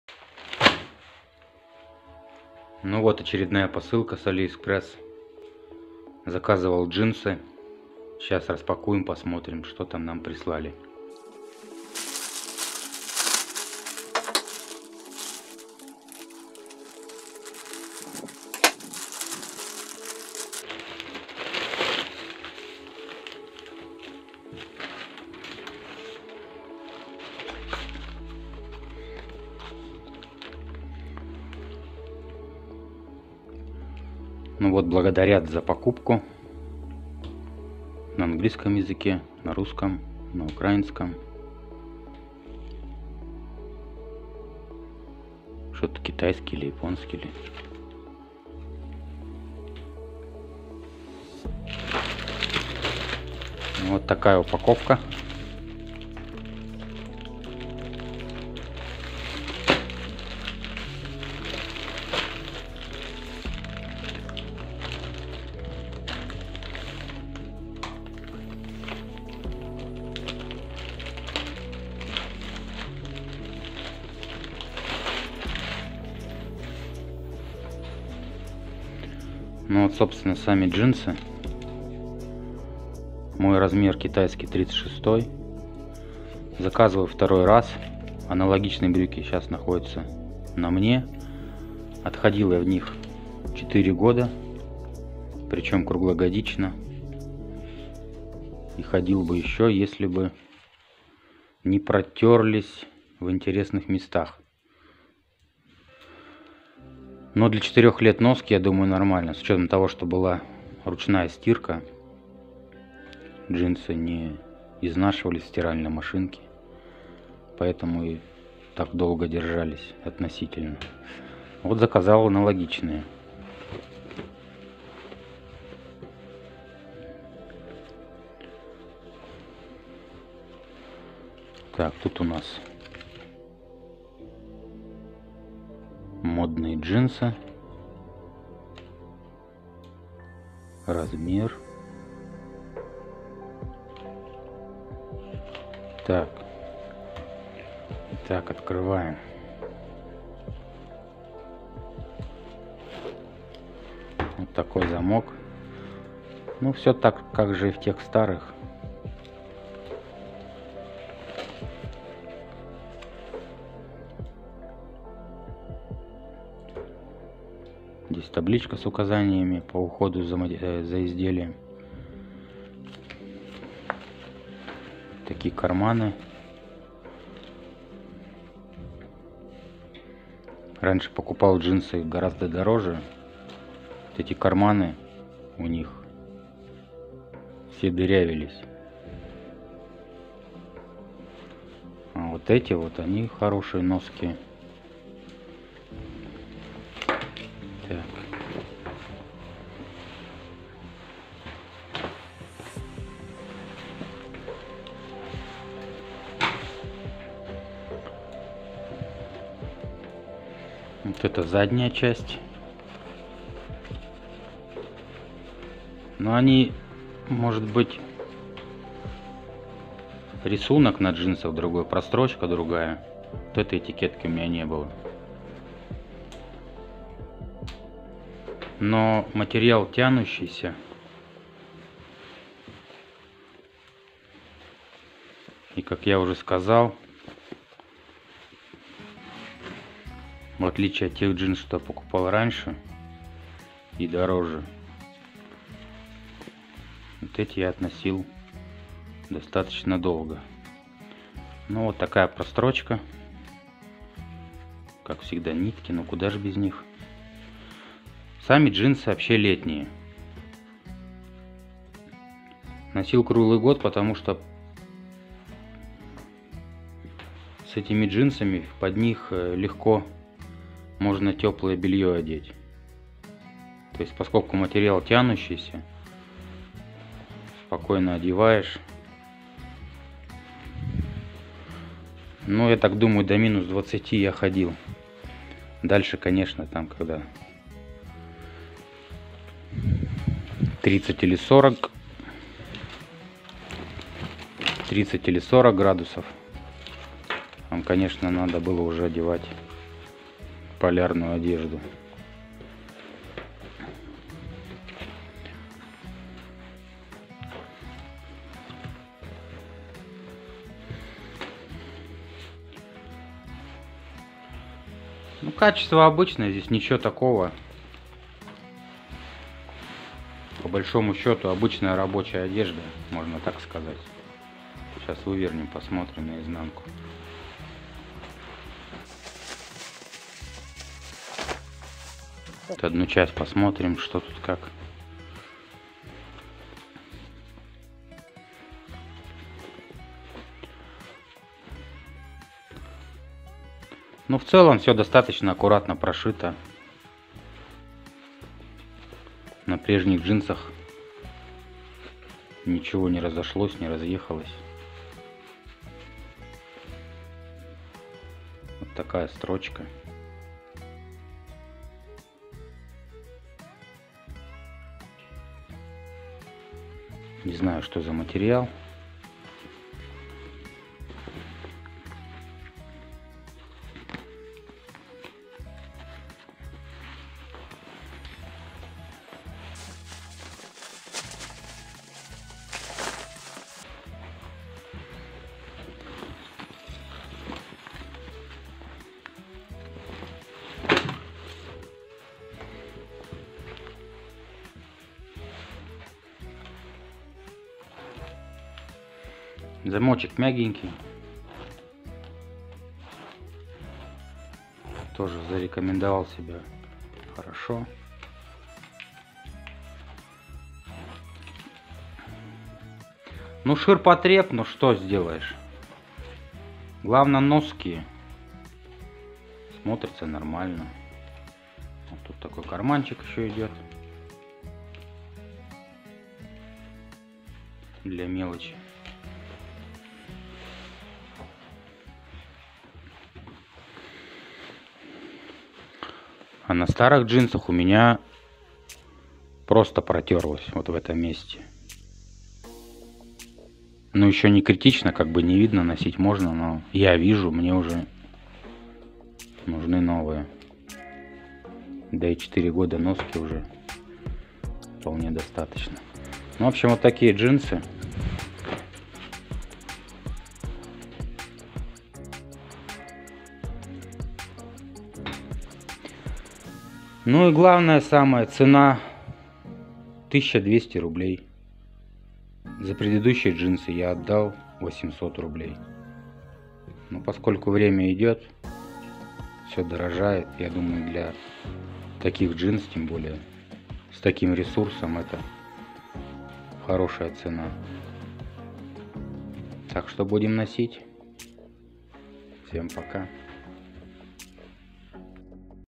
Распаковка посылки с алиэкспресс. Новые джинсы.
Посылка из китая.